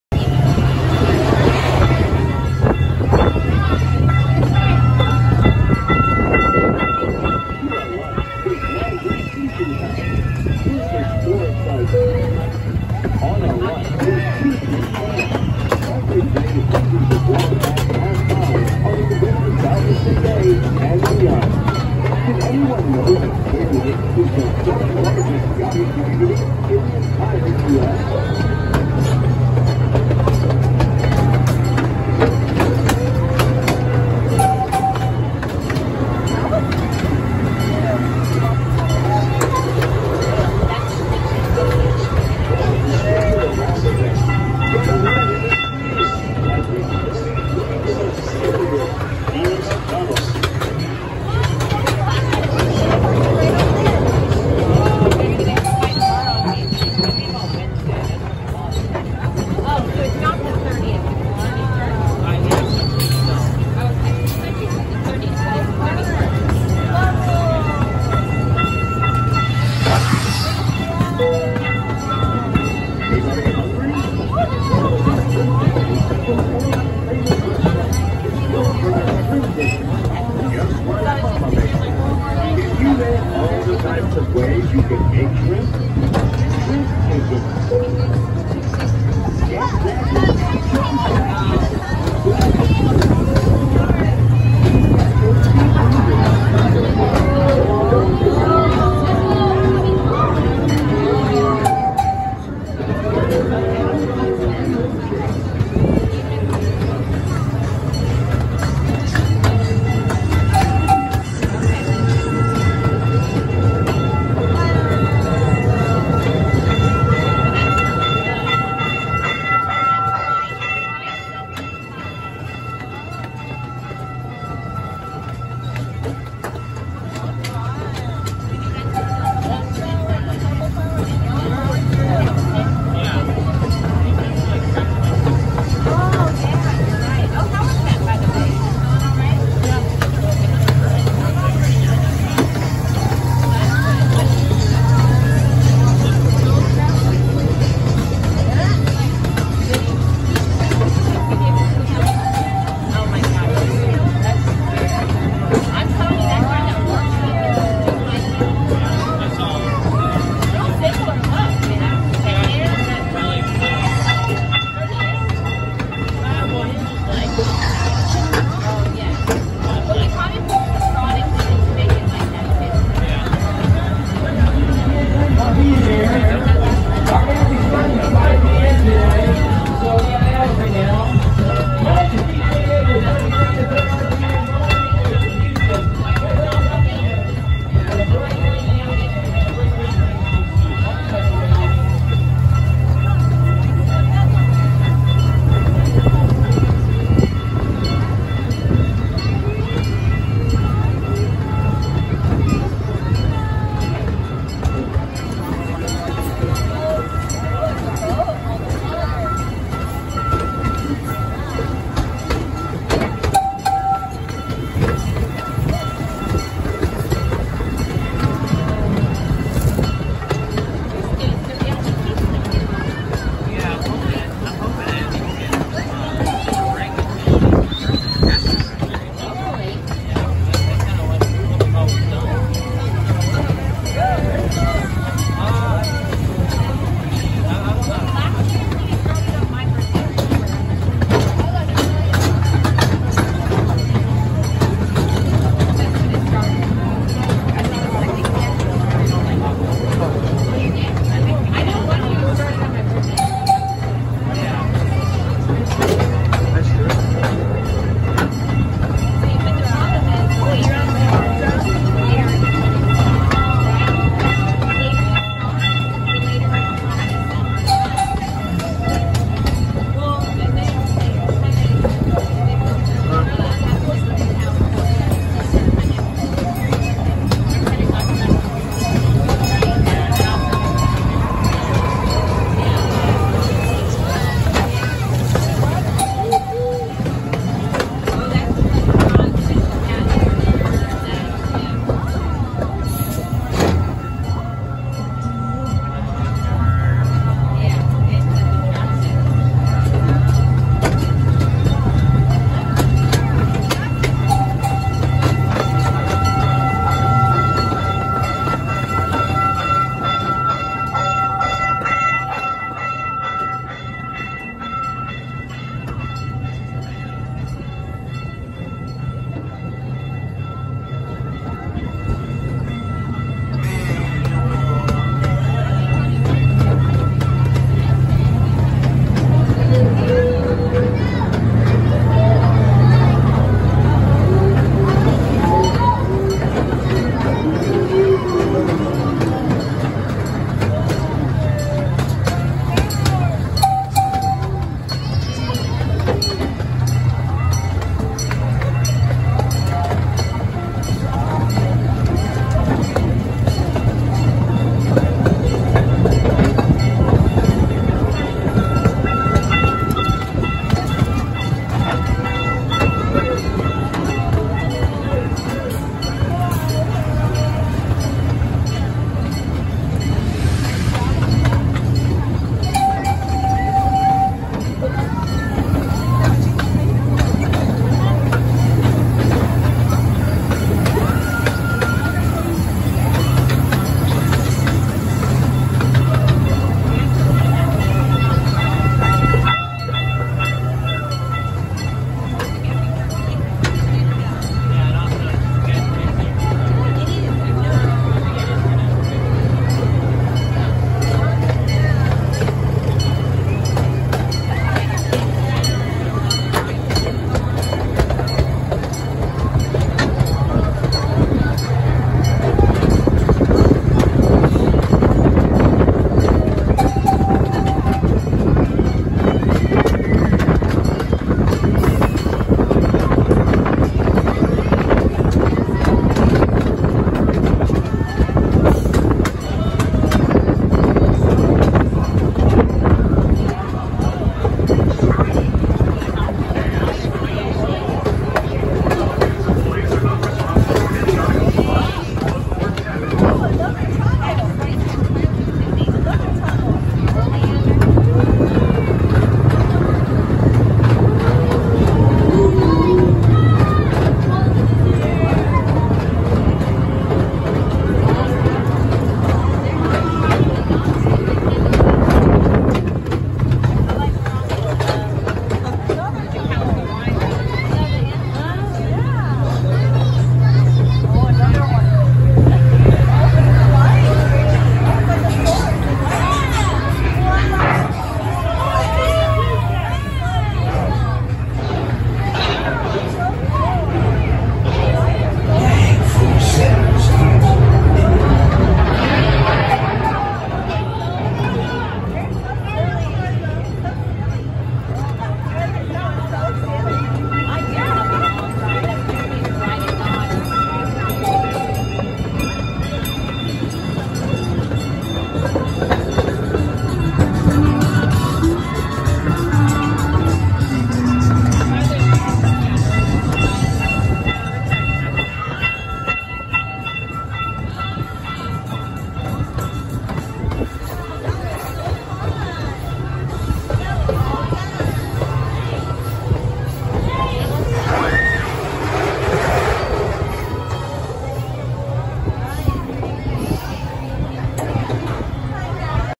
Kemah Train POV | Kemah Boardwalk | Kemah, Texas
This video shows the various views from riding the most popular ride at the Kemah Boardwalk -- its train around the property. There isn't a bunch of great things to see, but you can check of the restaurants, see the water, and see most of the attractions in the park. These are always a great hit with kids and families, and you can expect a long ride for the train (though, their nice wooden roller coaster should be a walk-on).

The Kemah Boardwalk is a small amusement park owned by Landry's in Kemah, Texas which is located near Space Center Houston and within the Houston-The Woodlands-Sugar Land Metropolitan area. Opening in 1997 to promote its various restaurants, such as Salt Grass and Joe's Crab Shack, the Kemah Boardwalk come onto Houstonian's and Coaster Enthusiasts maps in 2007 when they added Boardwalk Bullet, which, for most of its life, has been one of only three or four operating roller coasters in the Houston area.